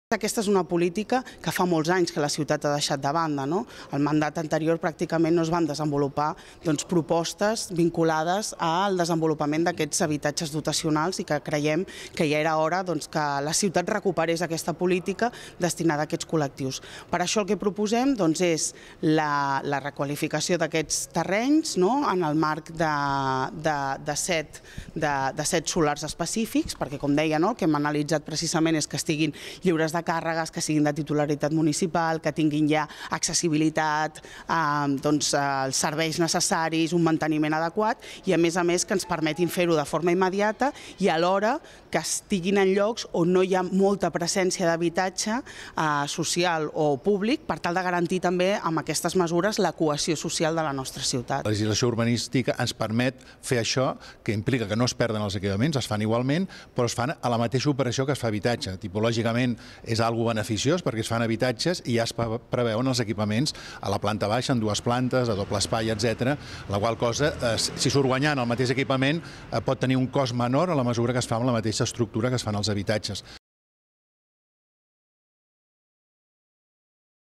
Josep Maria Montaner: "Els equipaments no es perden, es fan en la mateixa operació que l'habitatge"
(17/04/16) El regidor d'habitatge, Josep Maria Montaner, i la regidora d'Ecologia, Urbanisme i Mobilitat, Janet Sanz, han explicat que el Govern municipal ha donat un nou impuls a les polítiques de construcció d'habitatge dotacional que feia temps que estaven poc actives i que es tracta d'una iniciativa que inclourà habitatges nous i equipament municipal en els mateixos locals.